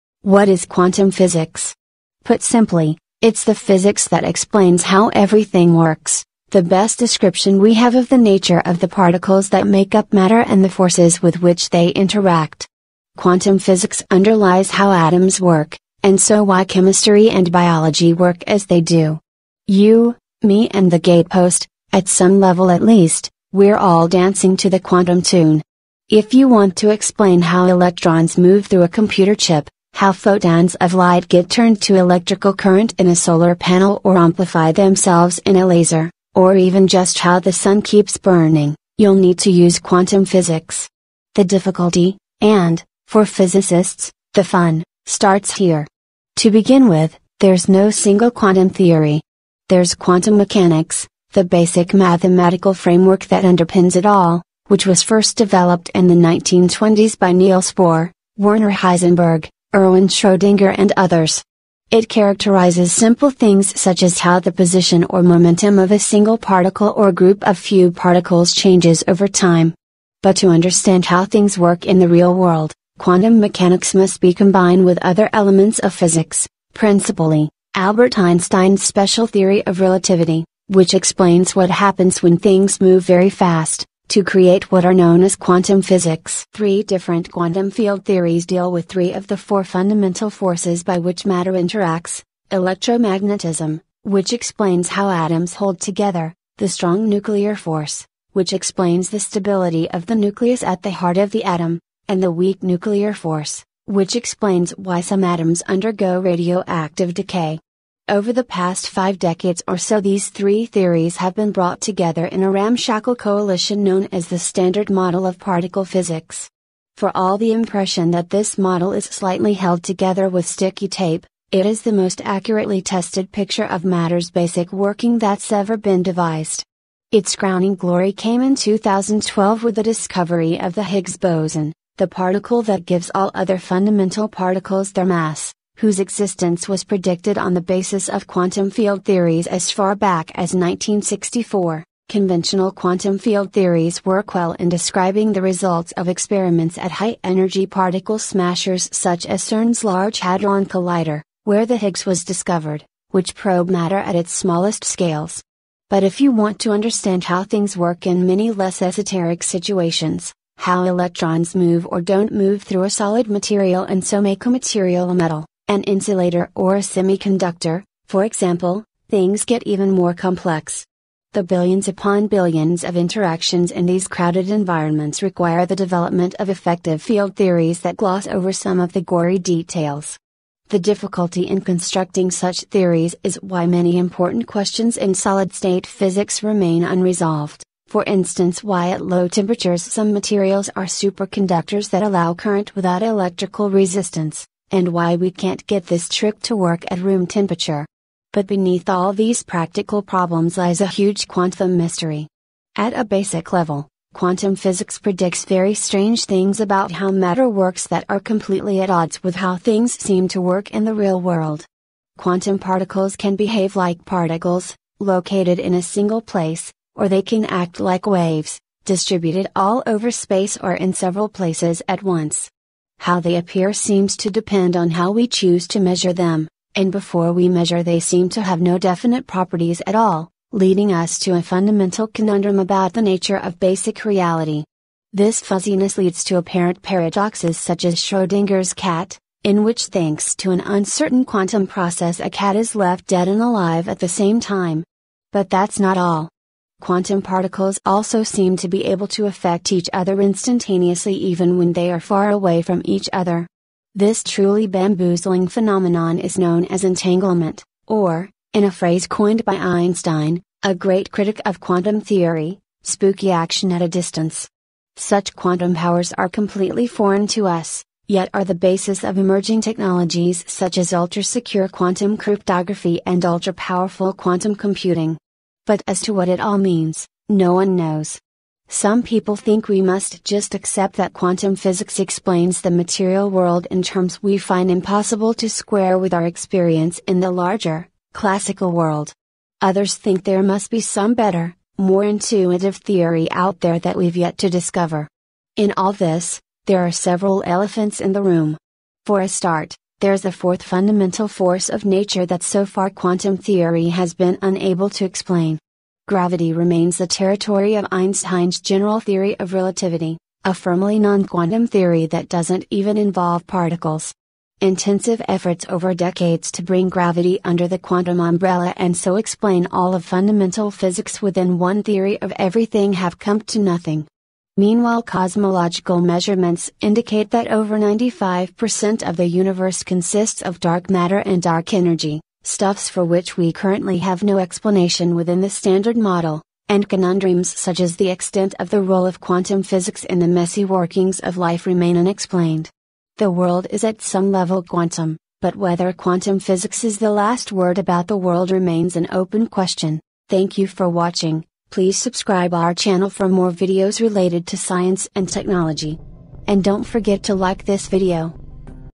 WHAT IS QUANTUM PHYSICS?
What is quantum physics? Put simply, it’s the physics that explains how everything works: the best description we have of the nature of the particles that make up matter and the forces with which they interact.

Quantum physics underlies how atoms work, and so why chemistry and biology work as they do. You, me and the gatepost – at some level at least, we’re all dancing to the quantum tune. If you want to explain how electrons move through a computer chip, how photons of light get turned to electrical current in a solar panel or amplify themselves in a laser, or even just how the sun keeps burning, you’ll need to use quantum physics.
The difficulty – and, for physicists, the fun – starts here. To begin with, there’s no single quantum theory. There’s quantum mechanics, the basic mathematical framework that underpins it all, which was first developed in the 1920s by Niels Bohr, Werner Heisenberg, Erwin Schrödinger and others. It characterises simple things such as how the position or momentum of a single particle or group of few particles changes over time.
But to understand how things work in the real world, quantum mechanics must be combined with other elements of physics – principally, Albert Einstein’s special theory of relativity, which explains what happens when things move very fast – to create what are known as quantum field theories.
Three different quantum field theories deal with three of the four fundamental forces by which matter interacts: electromagnetism, which explains how atoms hold together; the strong nuclear force, which explains the stability of the nucleus at the heart of the atom; and the weak nuclear force, which explains why some atoms undergo radioactive decay.
Over the past five decades or so these three theories have been brought together in a ramshackle coalition known as the “standard model” of particle physics. For all the impression that this model is slightly held together with sticky tape, it is the most accurately tested picture of matter’s basic working that’s ever been devised. Its crowning glory came in 2012 with the discovery of the Higgs boson, the particle that gives all other fundamental particles their mass, whose existence was predicted on the basis of quantum field theories as far back as 1964.
Conventional quantum field theories work well in describing the results of experiments at high-energy particle smashers such as CERN’s Large Hadron Collider, where the Higgs was discovered, which probe matter at its smallest scales. But if you want to understand how things work in many less esoteric situations – how electrons move or don’t move through a solid material and so make a material a metal, an insulator or a semiconductor, for example – things get even more complex.
The billions upon billions of interactions in these crowded environments require the development of “effective field theories” that gloss over some of the gory details. The difficulty in constructing such theories is why many important questions in solid-state physics remain unresolved – for instance why at low temperatures some materials are superconductors that allow current without electrical resistance, and why we can’t get this trick to work at room temperature.
But beneath all these practical problems lies a huge quantum mystery. At a basic level, quantum physics predicts very strange things about how matter works that are completely at odds with how things seem to work in the real world. Quantum particles can behave like particles, located in a single place; or they can act like waves, distributed all over space or in several places at once. How they appear seems to depend on how we choose to measure them, and before we measure they seem to have no definite properties at all – leading us to a fundamental conundrum about the nature of basic reality.
This fuzziness leads to apparent paradoxes such as Schrödinger’s cat, in which thanks to an uncertain quantum process a cat is left dead and alive at the same time. But that’s not all. Quantum particles also seem to be able to affect each other instantaneously even when they are far away from each other. This truly bamboozling phenomenon is known as entanglement, or, in a phrase coined by Einstein (a great critic of quantum theory), “spooky action at a distance”. Such quantum powers are completely foreign to us, yet are the basis of emerging technologies such as ultra-secure quantum cryptography and ultra-powerful quantum computing.
But as to what it all means, no one knows. Some people think we must just accept that quantum physics explains the material world in terms we find impossible to square with our experience in the larger, “classical” world. Others think there must be some better, more intuitive theory out there that we’ve yet to discover.